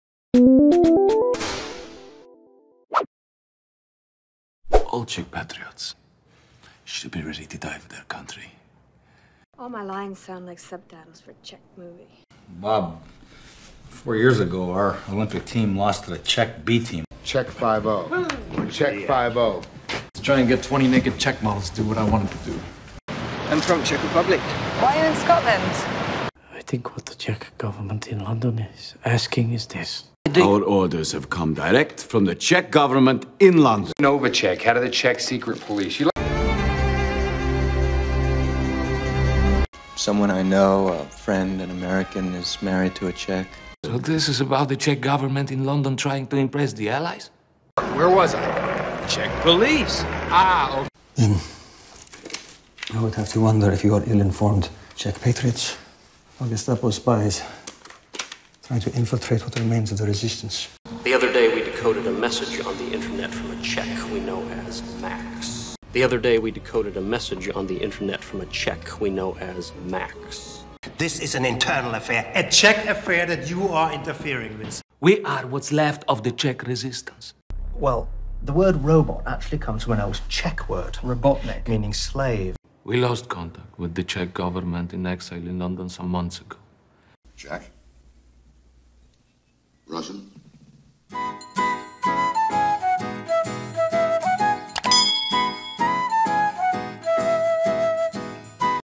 🇨🇿 Every CZECH REPUBLIC Mention In The Movies
We have analyzed every movie (probably not) to find each CZECH REPUBLIC mention. Can you guess all of the movies?